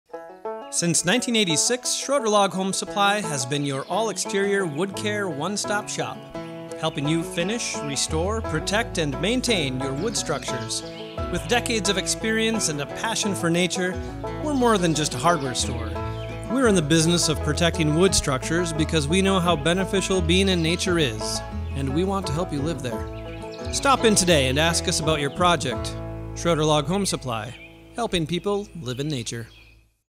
Schroeder Log Home Supply - Helping People Live in Nature
We furnish the legacy of log, timber, and wood structures.

Since 1986, we’ve been helping people live in nature by furnishing the legacy of log structures; offering quality products complemented with contractor referrals and DIY advice. We stock caulking, chinking, log finishes, tools, wood treatments, and restoration products for log homes.